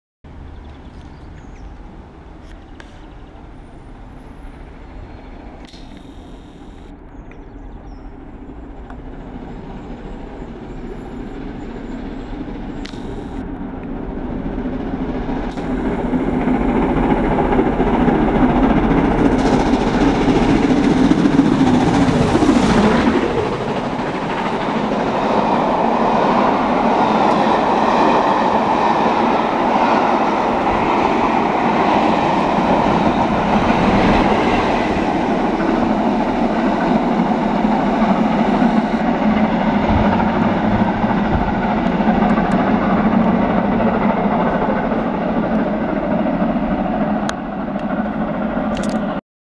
The Duchess of Sutherland
The Duchess of Sutherland at Bushy Station near Watford